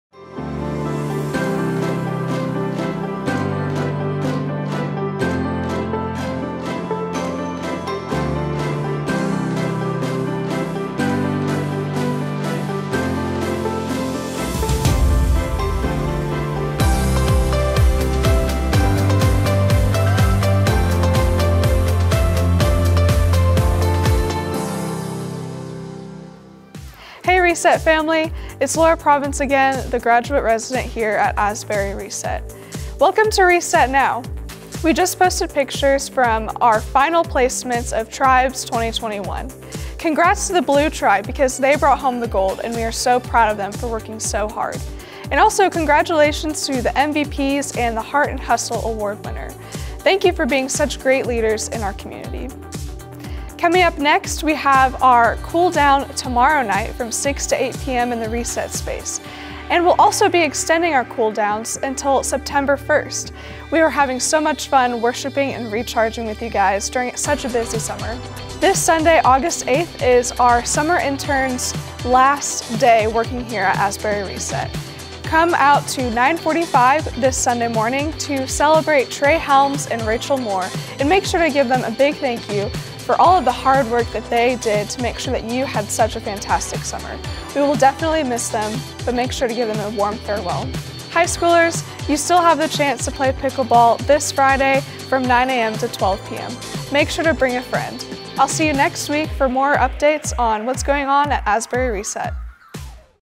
RESET News, Week of August 8, 2021
Congrats to the Blue Tribe! They brought home the gold on Sunday in the final Tribes of 2021. See you at Cool Down on Wednesday, August 4 from 6-8:00 p.m. Then, on Sunday, August 8, help us say farewell to our summer interns, Trey and Rachel.

To learn more about the student ministry at Asbury United Methodist Church in Birmingham, AL,